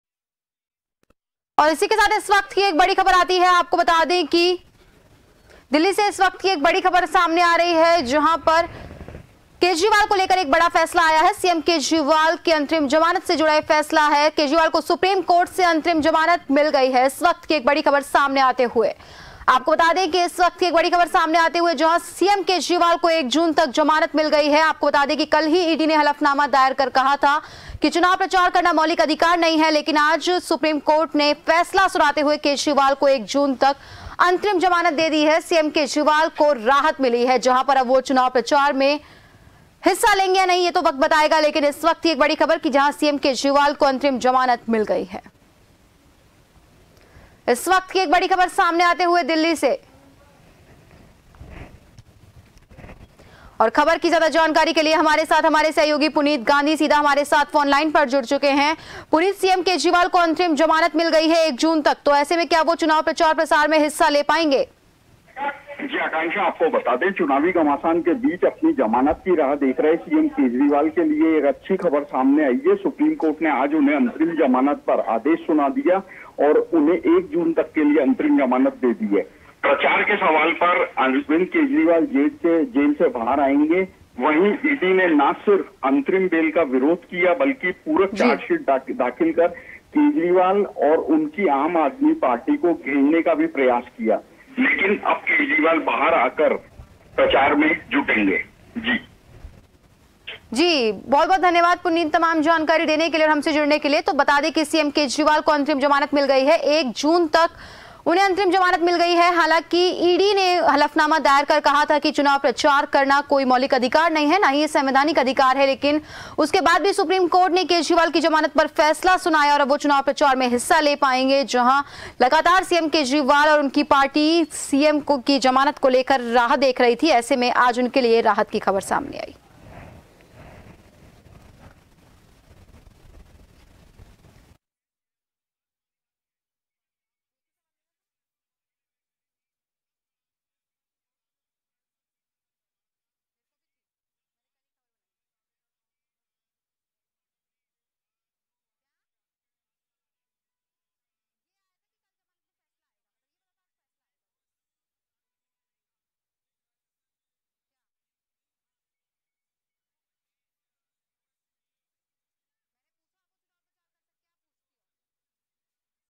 Delhi News | दिल्ली-सीएम केजरीवाल को अंतरिम जमानत, सीएम केजरीवाल को 1 जून तक अंतरिम जमानत | JAN TV
Watch JAN TV on :
Tata Play DTH : 1185
Airtel DTH: 355
JIO Fiber: 1384